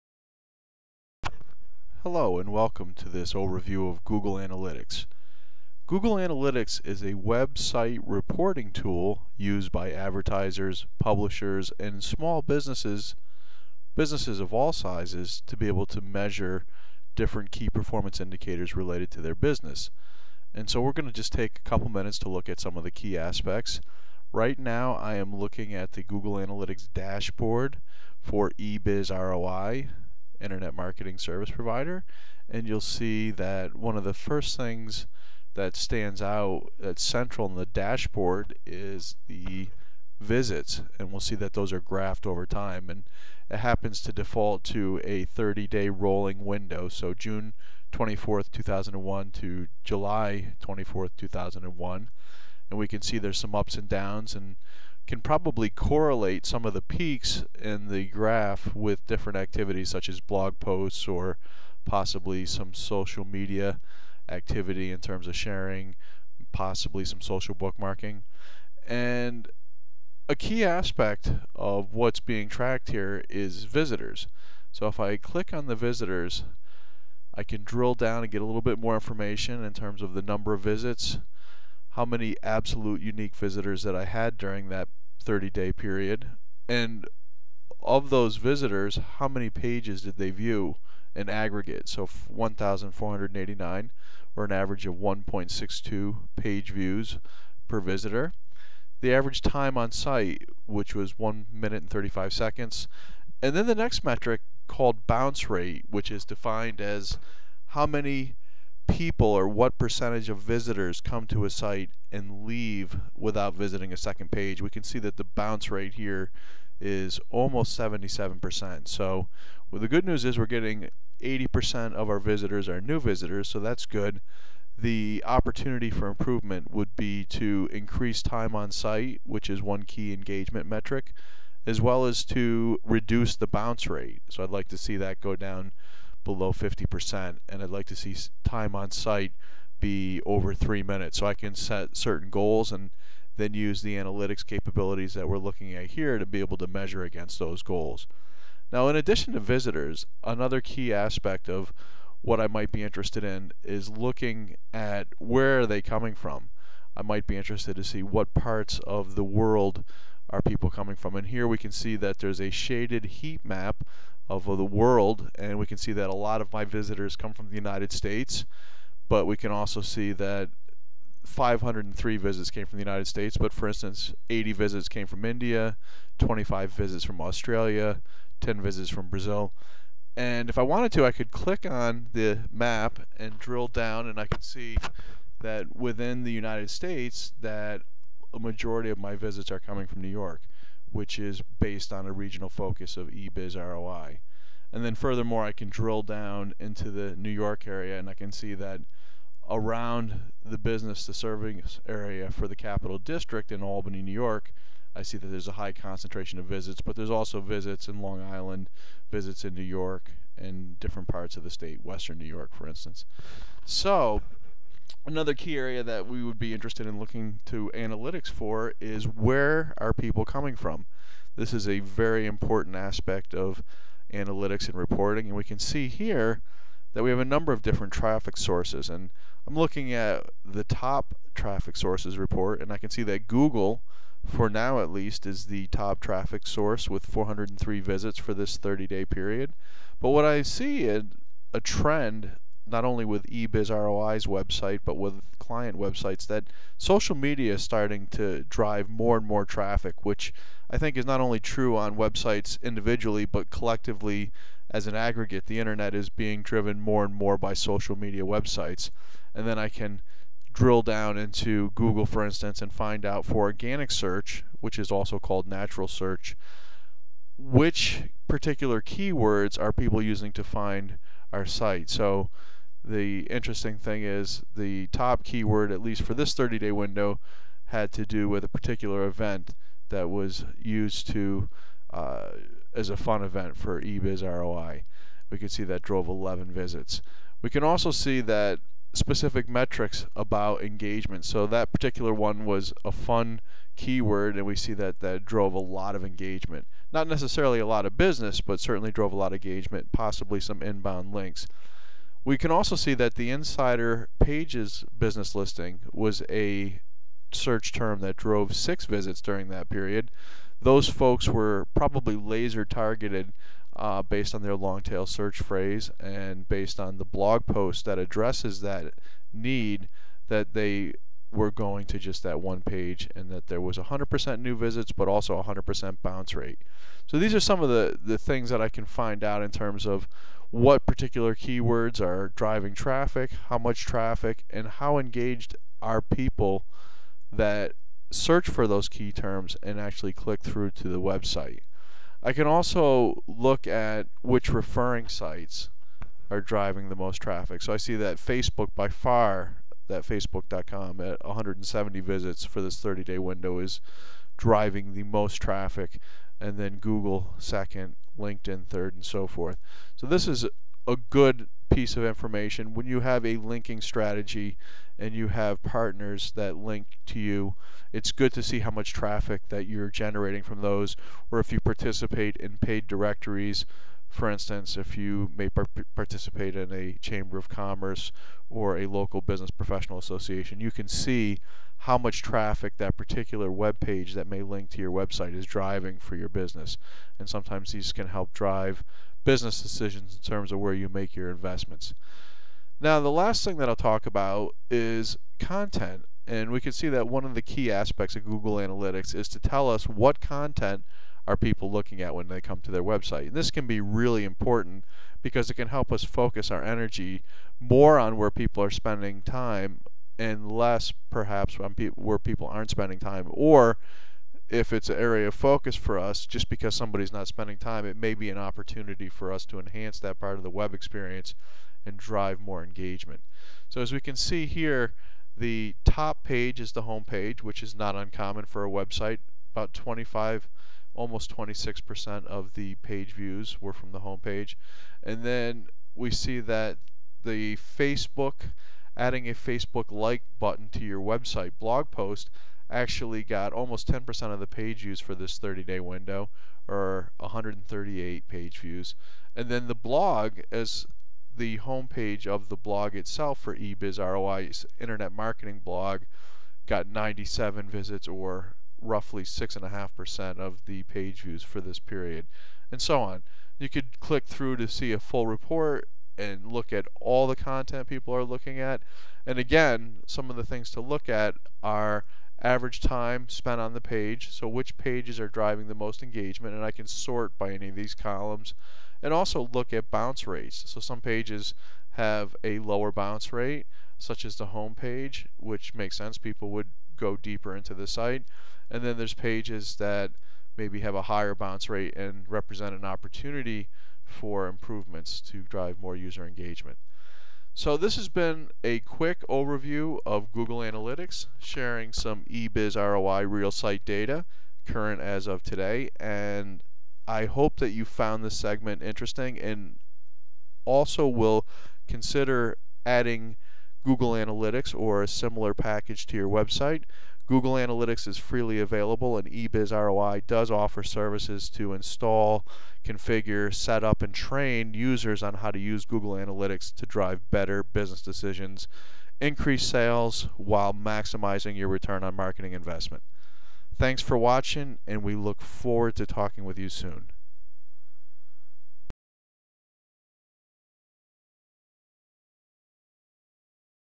Google Analytics Demo
Google Analytics Demo provide by eBiz ROI, Inc. An overview of the Google Analytics interface is provided along with key metrics using real data from the website. Key performance indicators reviewed include bounce rate (only visit one page then leave), average time on site, average pages per visit, unique visitors and other important user engagement metrics. Great overview for individual or organization that is interested in measuring and tracking their website ROI while harvesting insights on how the website data tracked can be analyzed to find areas of opportunity for improvement to increase sales and marketing ROI.